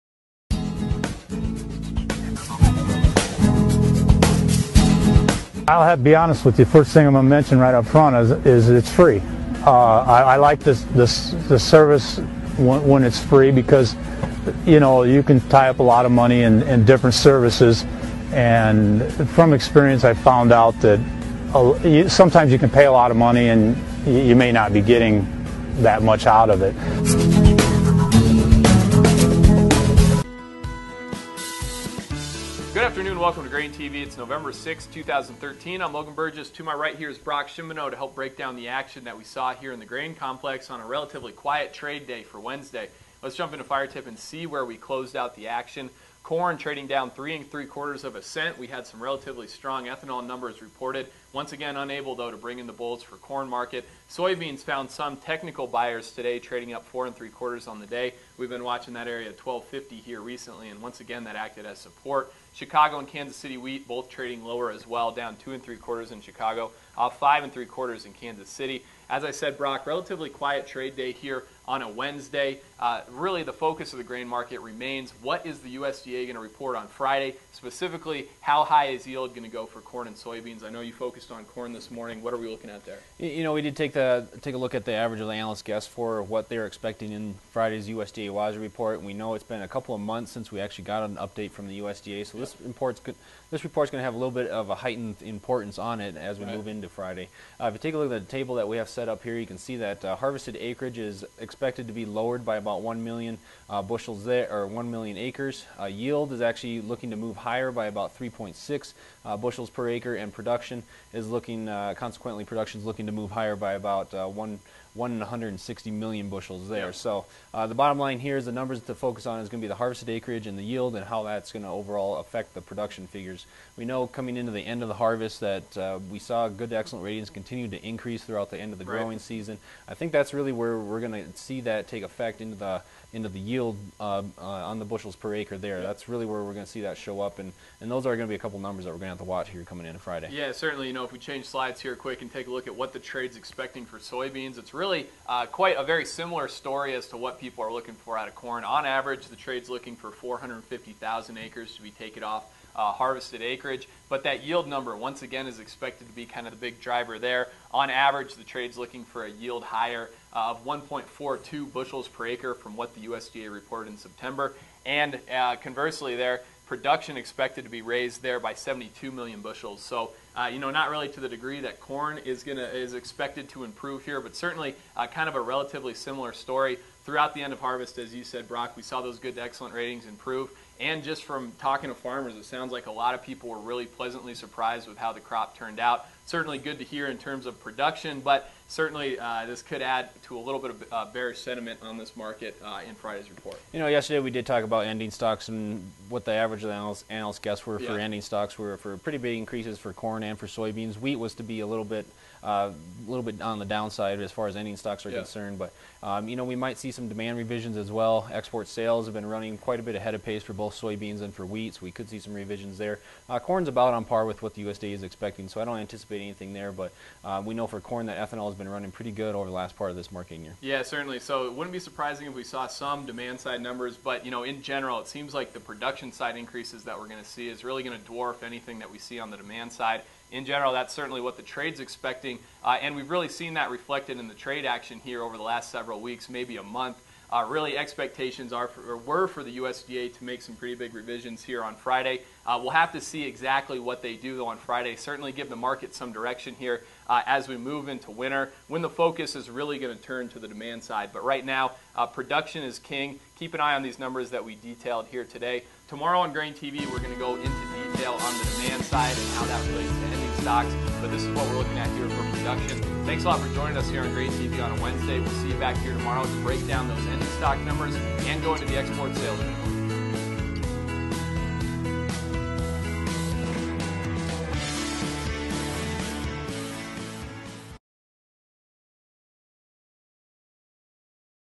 GrainTV - November 6th, 2013
Tune in to see Brock and Logan discuss corn and soybean production.